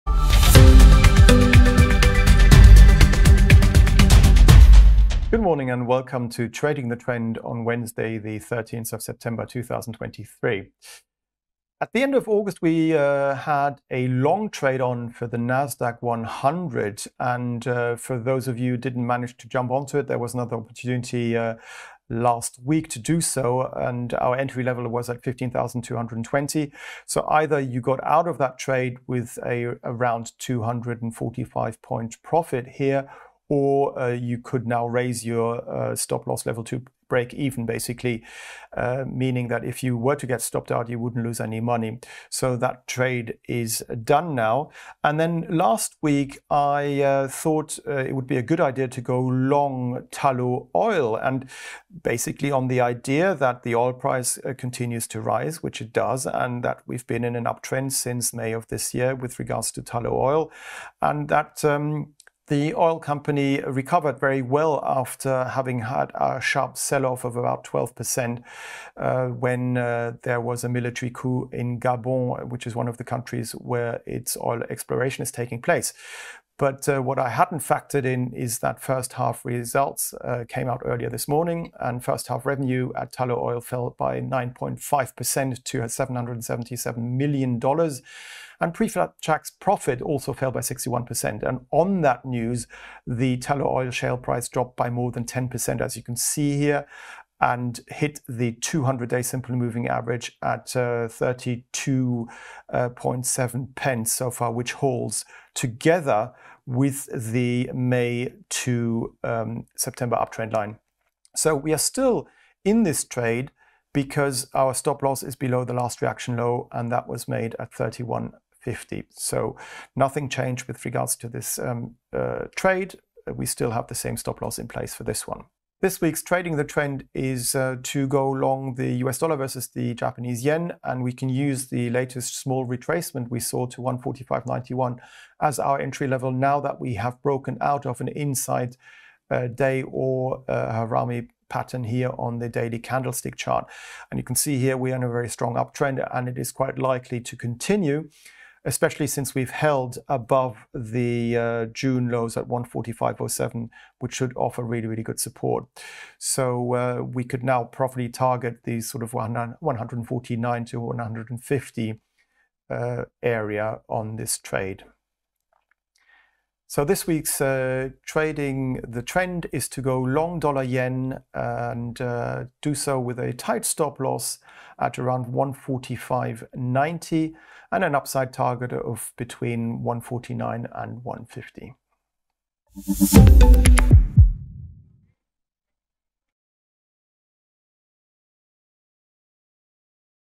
Trading the Trend - 13 September 2023: long USD/JPY
Trading the Trend: long USD/JPY

USD/JPY has resumed its uptrend and Axel Rudolph would like to go long with an upside target of between ¥149 to ¥150 and a stop loss at ¥145.90.